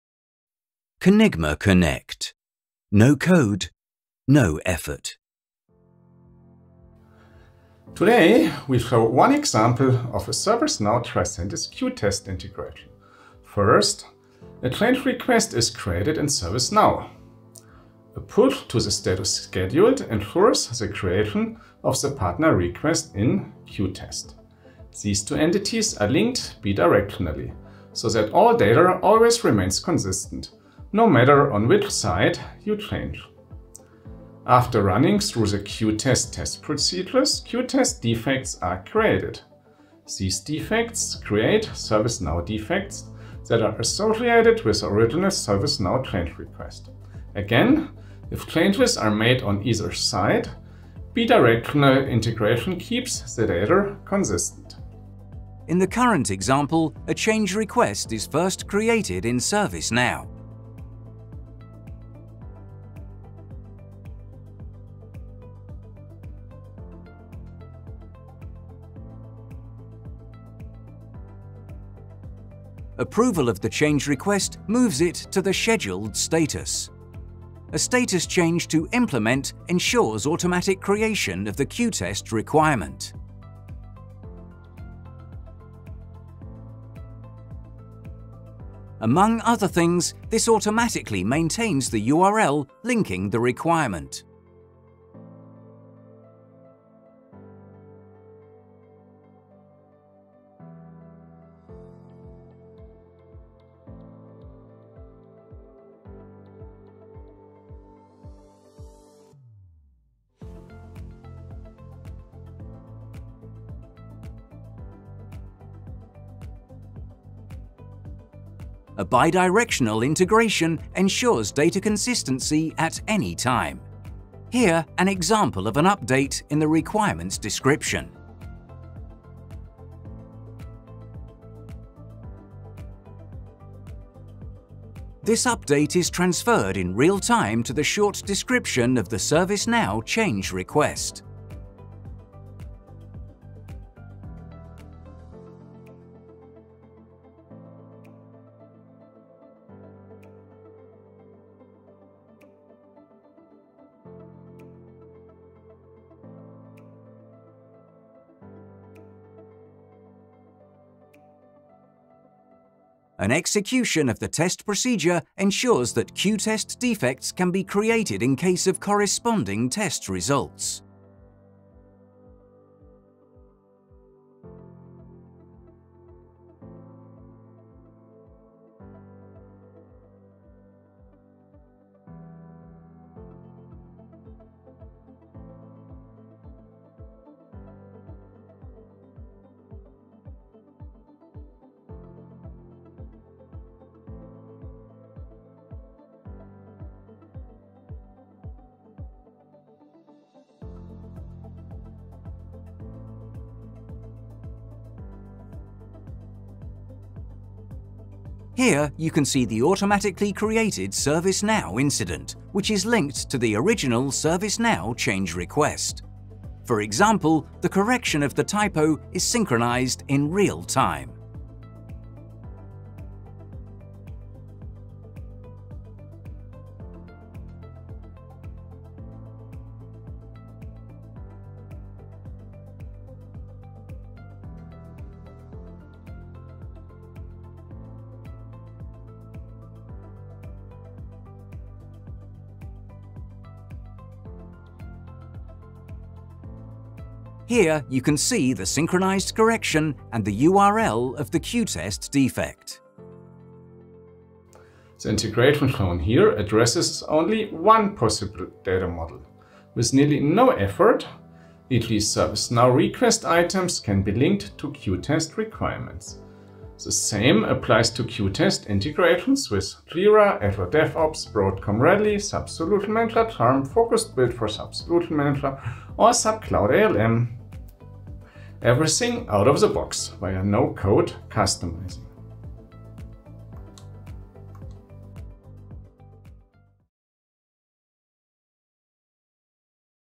Bidirectionally connect Tricentis qTest with ServiceNow
🚀This video shows an example of a ServiceNow - Tricentis qTest integration.
First, a change request is created in ServiceNow. A push to the status "Scheduled" ensures the creation of the partner request in qTest. These two entities are linked bidirectionally, so that all data always remains consistent, no matter on which side you change.
After running through the qTest test procedures, qTest defects are created.
These defects create ServiceNow defects that are associated with the original ServiceNow change request.  Again, if changes are made on either side, bidirectional integration keeps the data consistent.

🚀The integration shown here addresses only one possible data model. With nearly no efferot, e.g. ServiceNow Request Items can be linked to qTest Requirements. The same applies to qTest integrations with Jira, Azure DevOps, Broadcom Rally, SAP Solution Manager ChaRM, Focused Build for SAP Solution Manager or SAP Cloud ALM.

✅Everything out-of-the-box via no-code customizing.